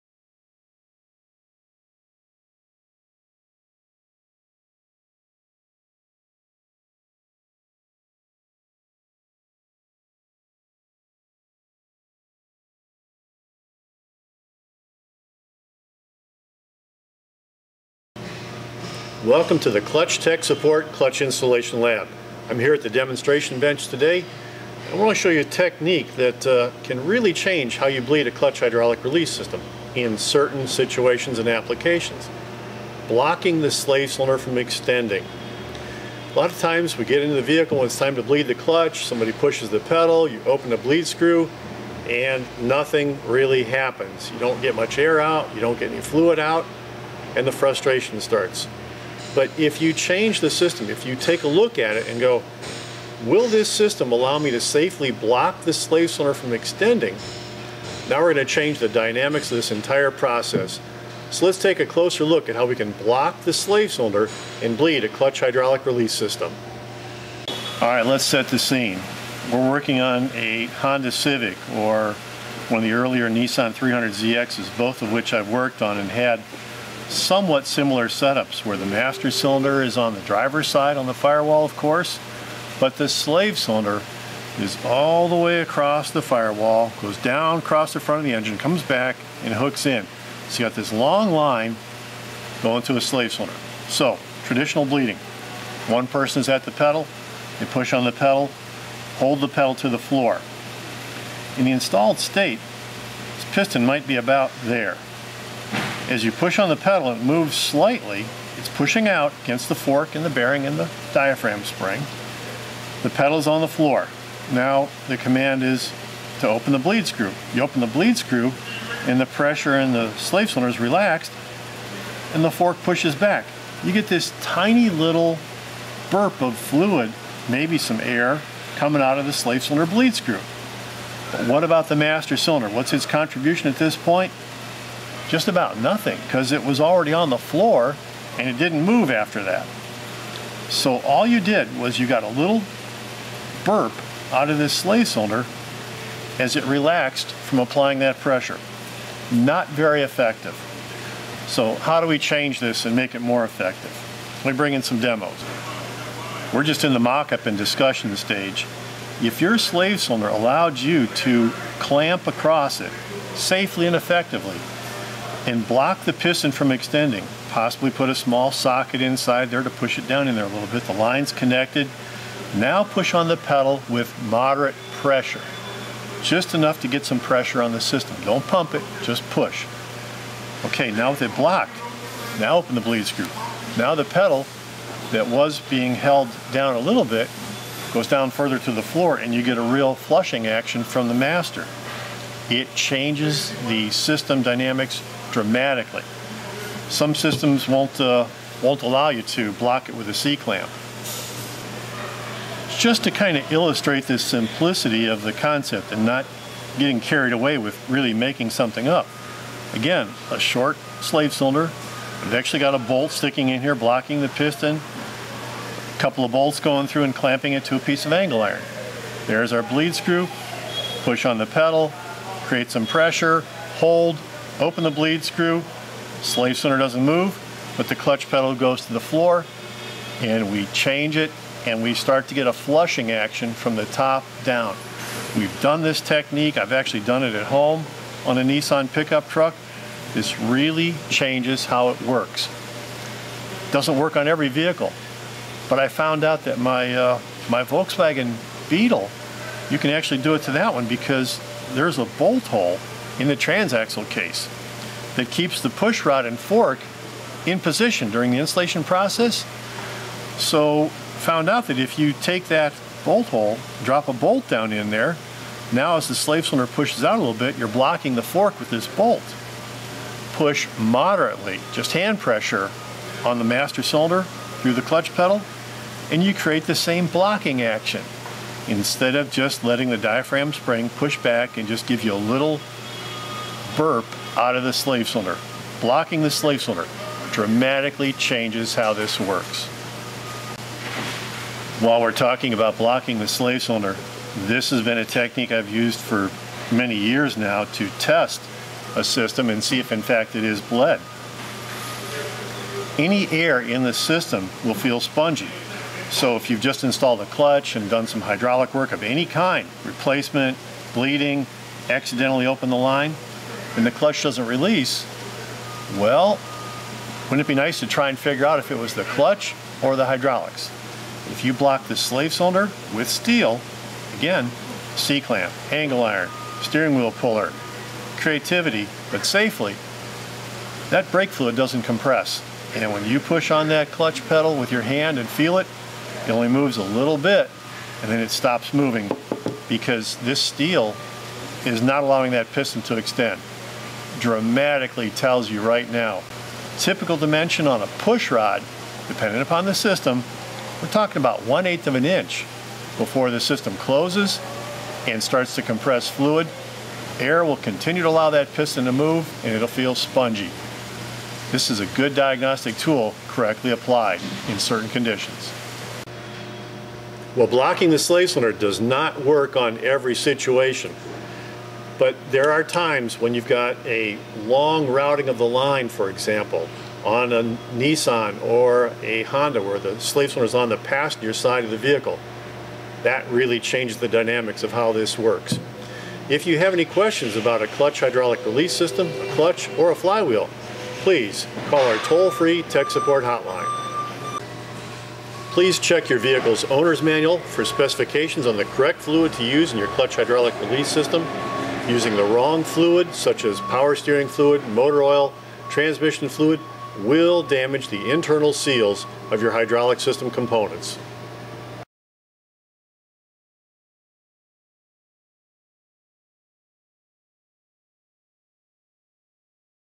Bleeding a Clutch by Blocking the Slave Cylinder
Blocking a clutch slave cylinder allows the clutch master cylinder to actually start flushing from the master cylinder down to bleed a clutch hydraulic system. This can only work with external slave cylinders that can safely and effectively be blocked. For instruction bulletins and additional videos by our part number go